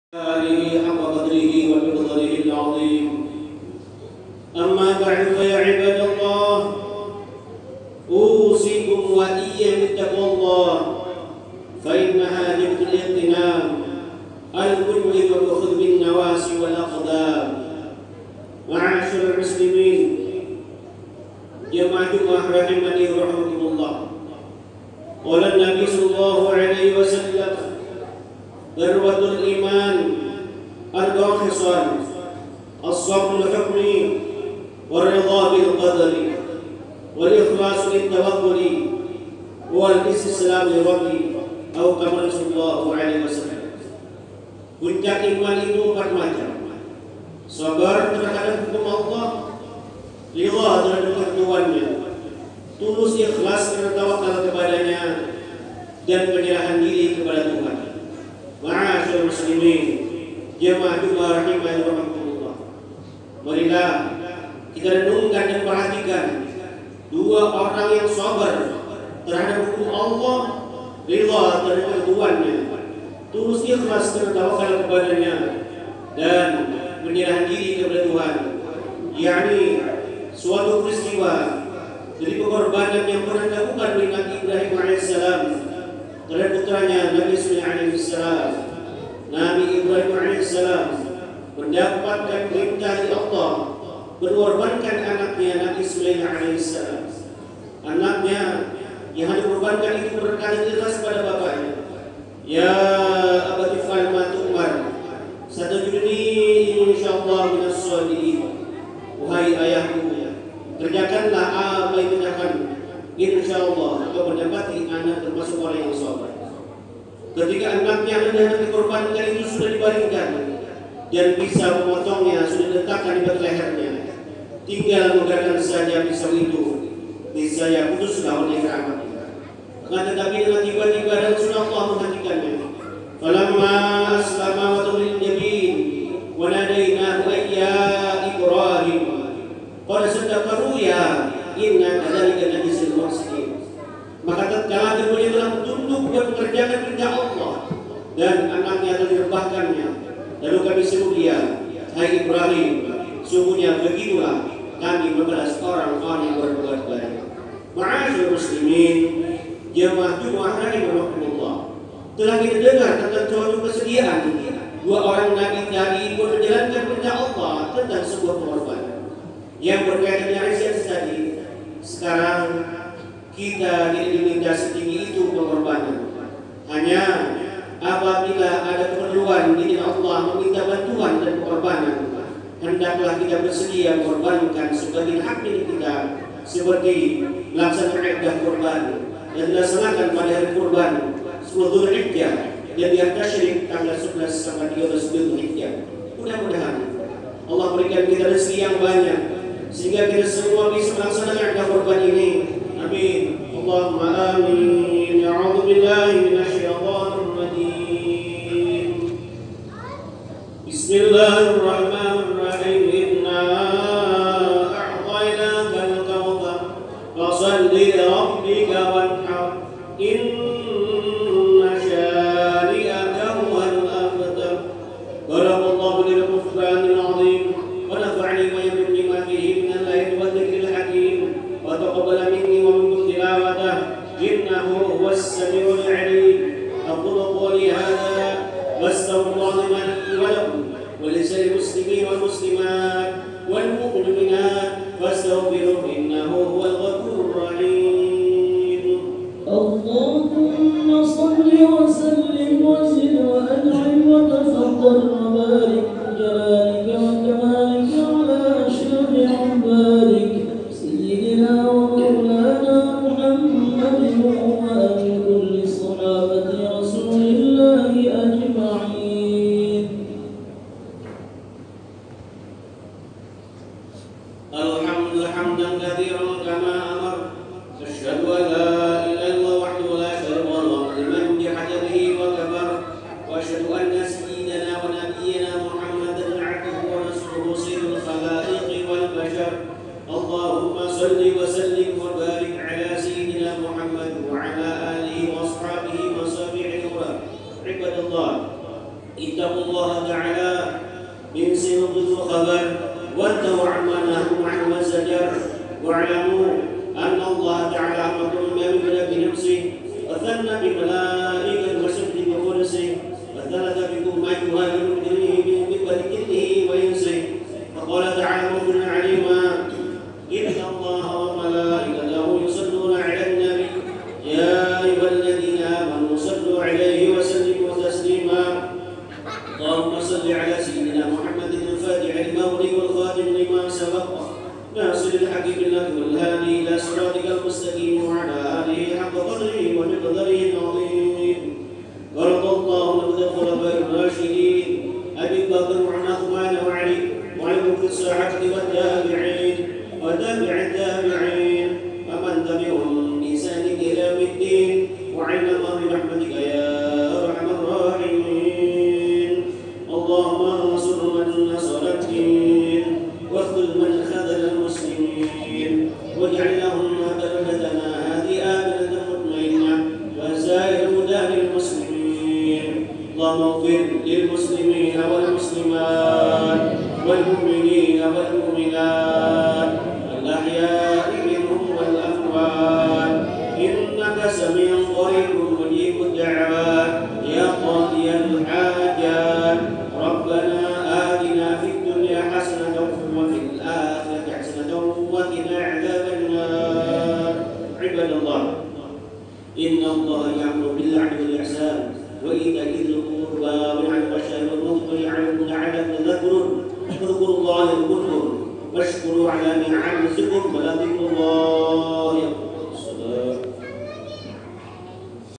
Khutbah Jum'at di Masjid Nurul Iman 9 Agustus 2019
INFORMASI KAPUAS - Direkam di Jalan Kapten Pierre Tendean Gg. VIII, Kuala Kapuas, Kalimantan Tengah.